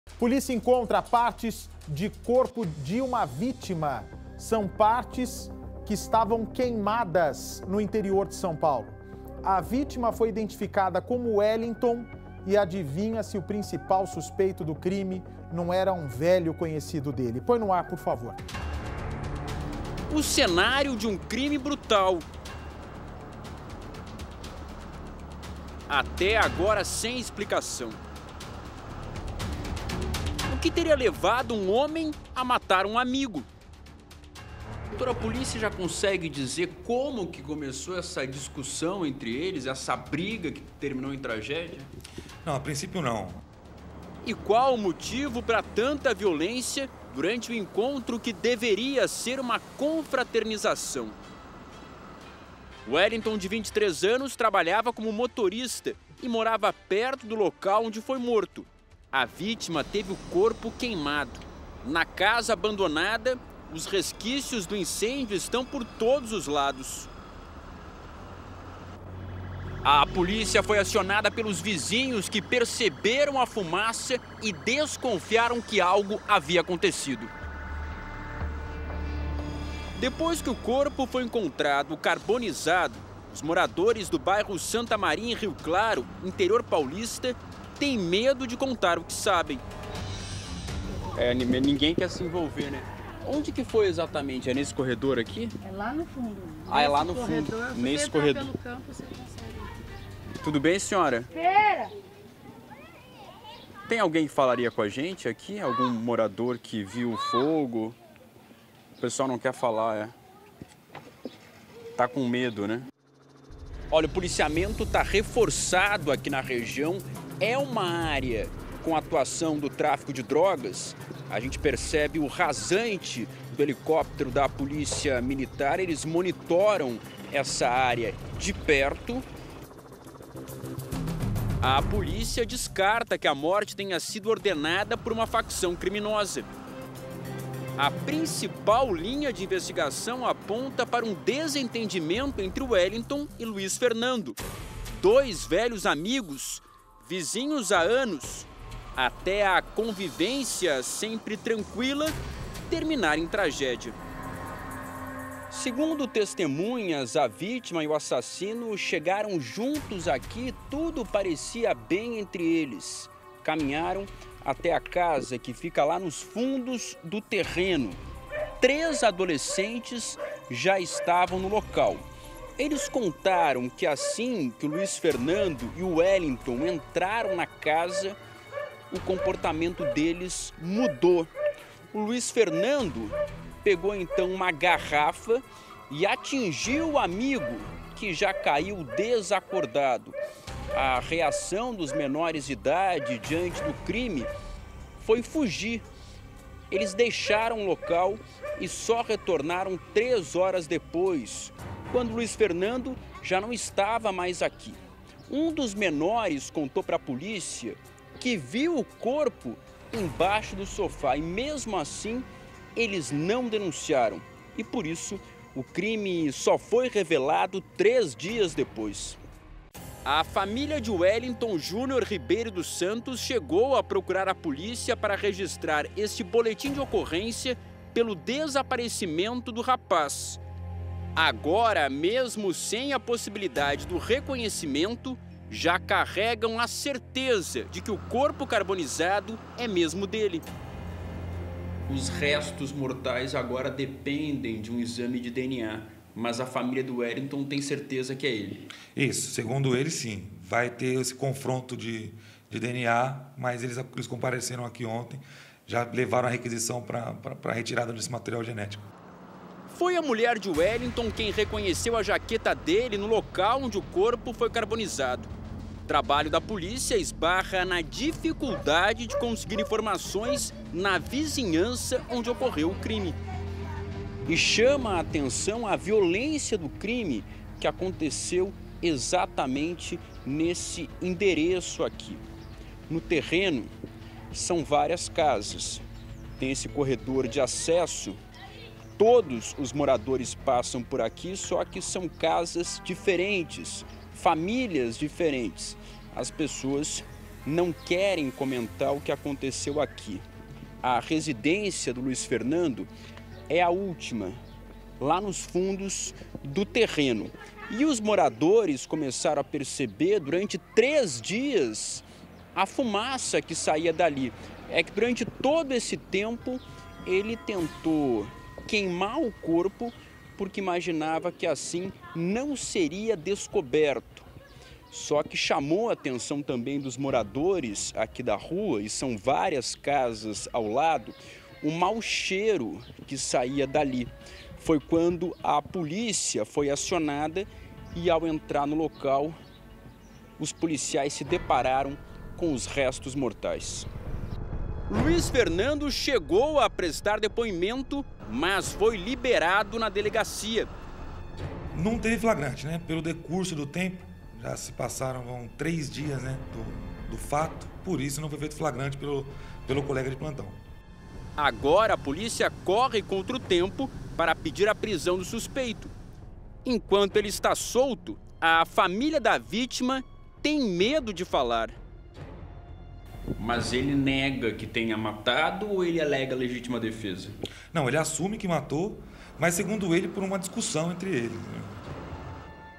Polícia encontra corpo em quintal de casa; amigo confessa assassinato
Em Rio Claro, no interior de São Paulo, a polícia encontrou partes de corpo queimadas no quintal de uma casa. O principal suspeito é um amigo da vítima. Ele confessou o crime na delegacia e, mesmo assim, saiu pela porta da frente em liberdade. A polícia agora tenta juntar provas para pedir a prisão do assassino.